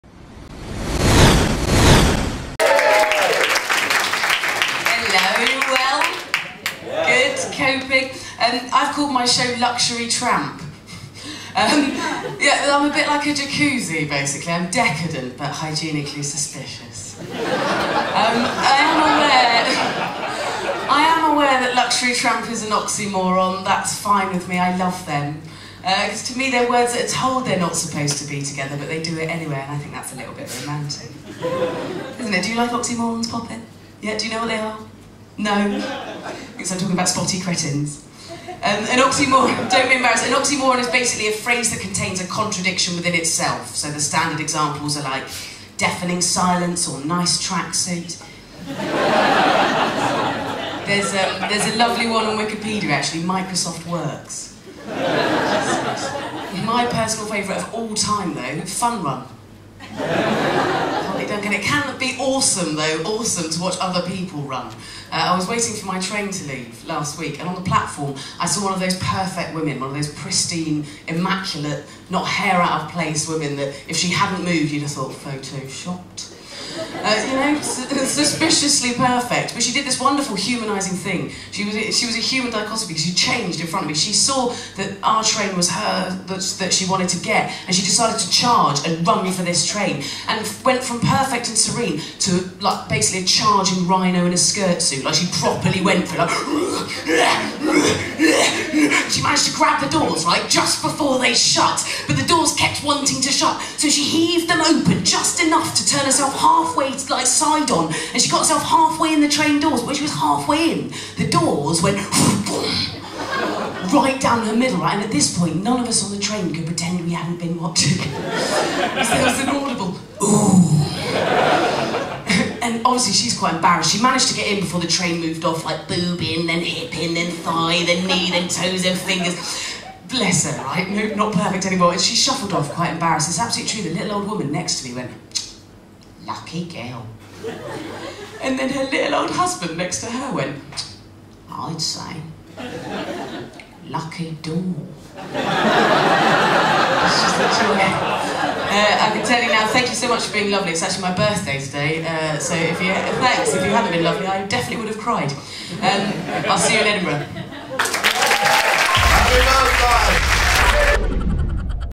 Jessica Fostekew at Chortle's Fast Fringe
With an extract from her Edinburgh show Luxury Tramp